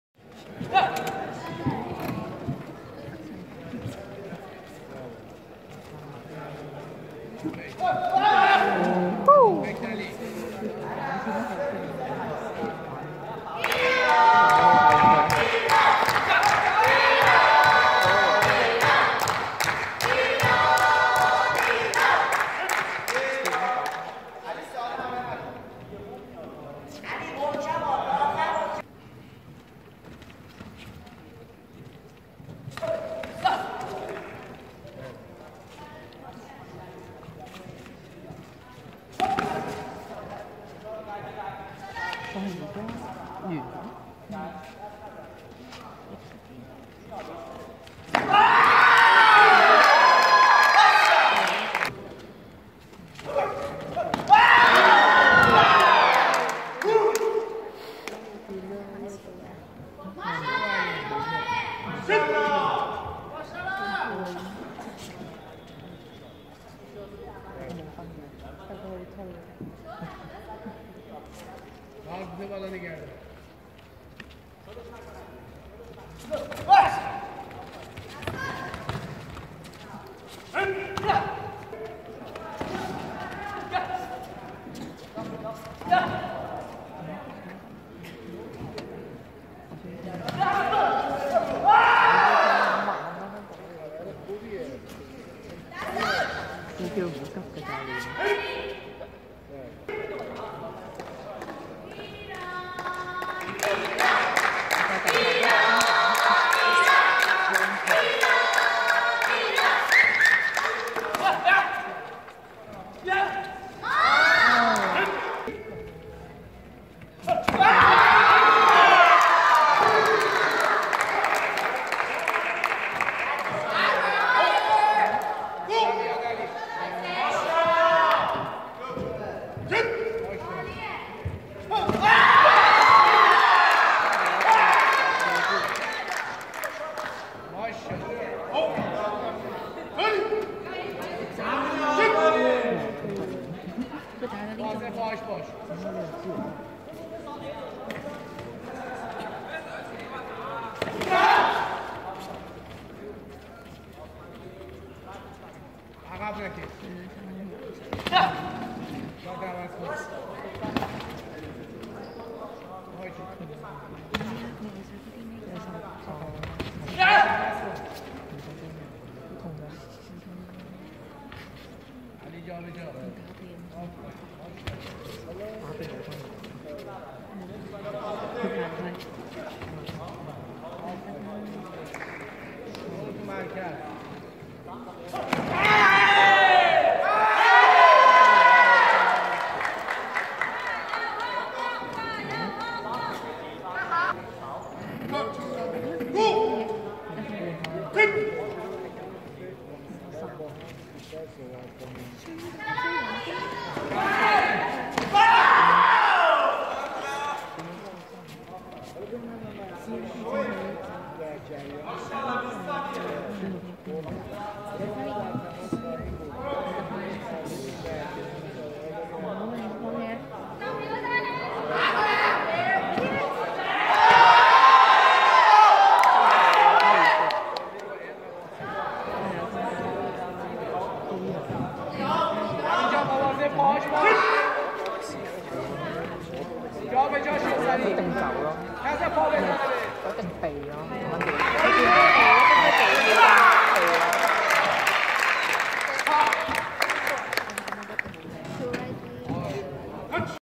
1st AUKC 2019_ Meskini Ali (Iran) vs Him Chau Ka (China)
Male Individual Kumite below 60kg
Final bout

1st Asian University Karate Championship 2019 (Macau, China)